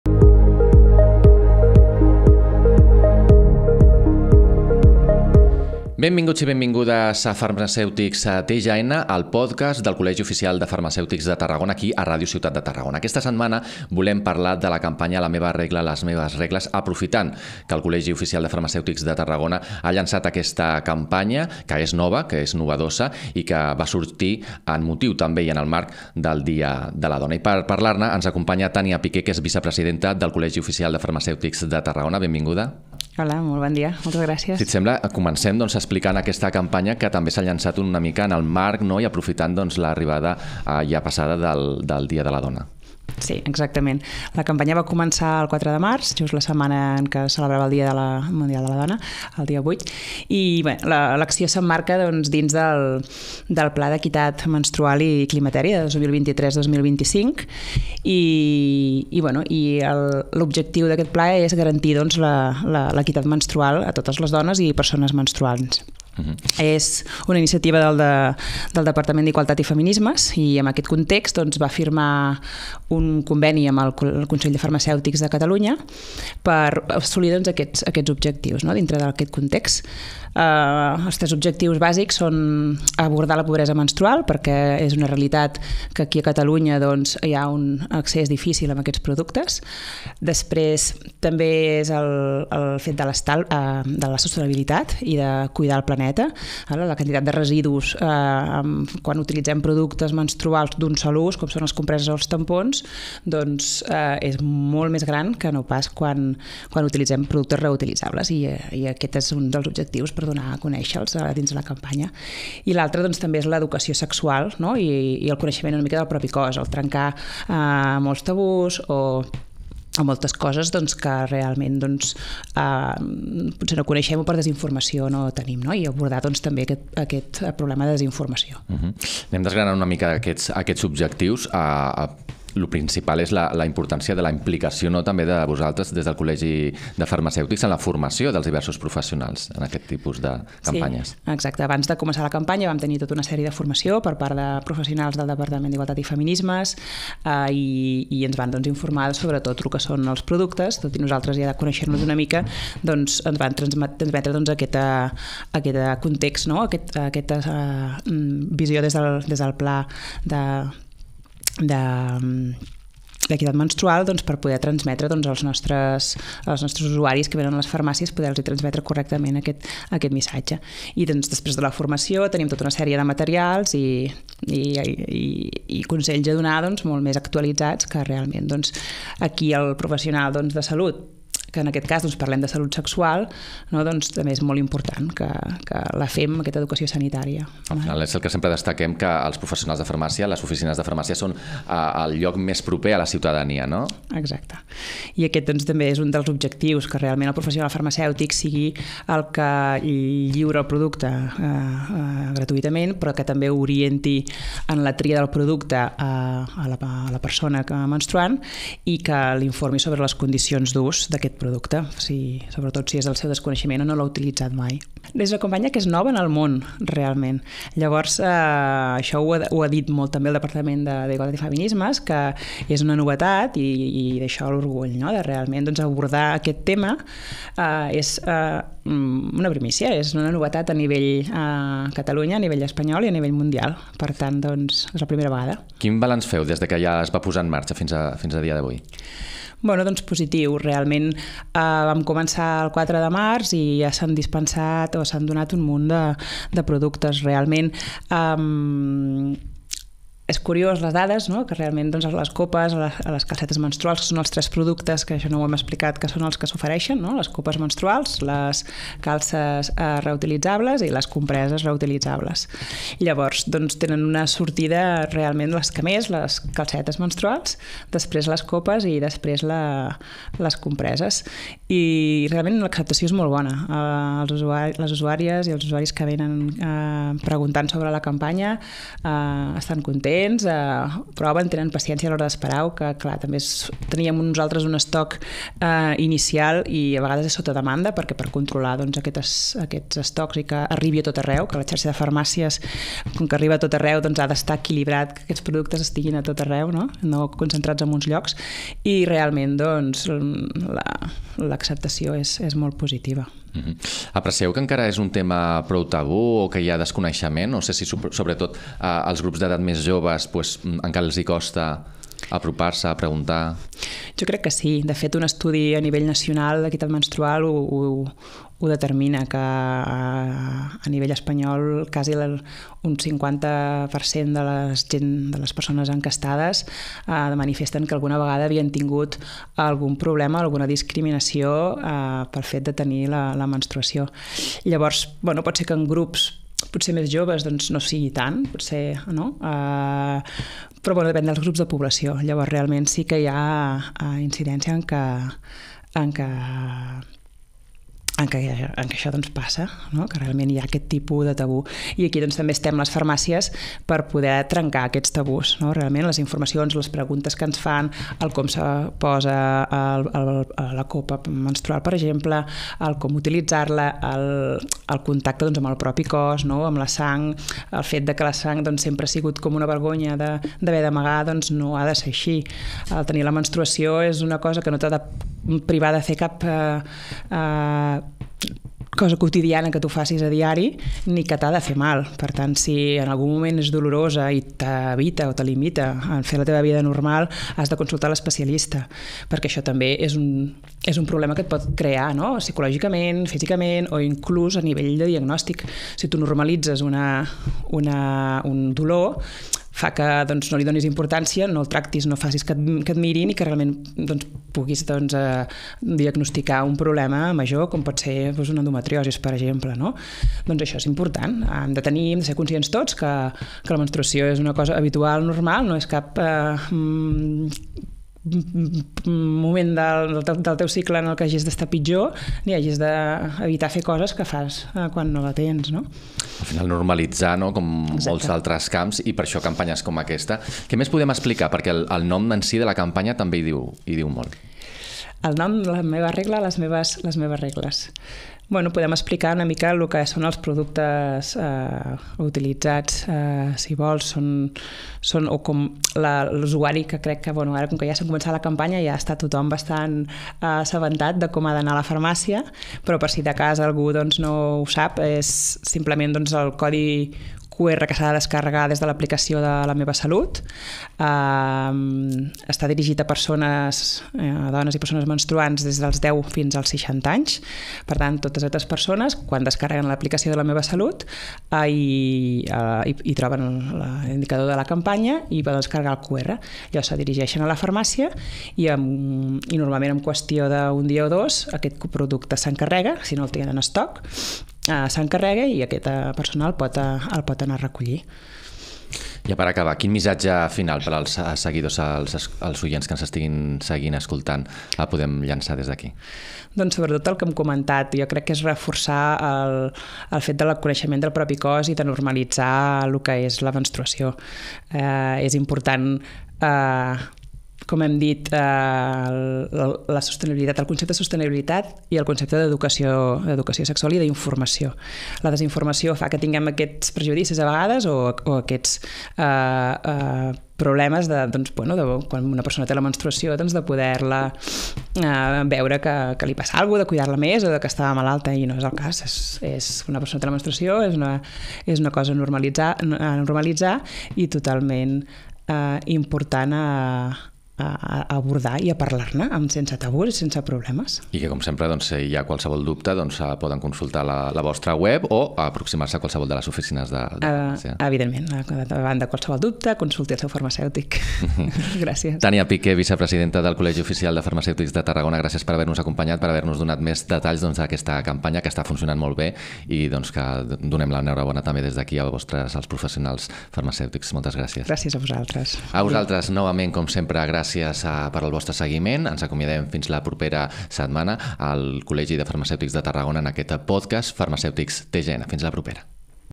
FarmacèuticsTGN | Campanya 'La meva regla, les meves regles'
La vicepresidenta del Col·legi Oficial de Farmacèutics de Tarragona, Tània Piqué, ens parla, detalla i valora la campanya 'La meva regla, les meves regles' en aquest nou capítol del podcast

Totes les dones i persones menstruants han de poder decidir lliurement i amb la informació adequada com volen gestionar el seu cicle menstrual. Per això, la Generalitat de Catalunya promou i garanteix l’accés als productes reutilitzables i sostenibles mitjançant la seva distribució gratuïta.

El dret a l’equitat menstrual comprèn l’accés a l’educació sobre el propi cos i la salut amb perspectiva de gènere, com també l’accés als productes menstruals adequats per a cada persona.

És una campanya pionera del Departament d’Igualtat i Feminismes i el Consell de Col·legis Farmacèutics de Catalunya (CCFC)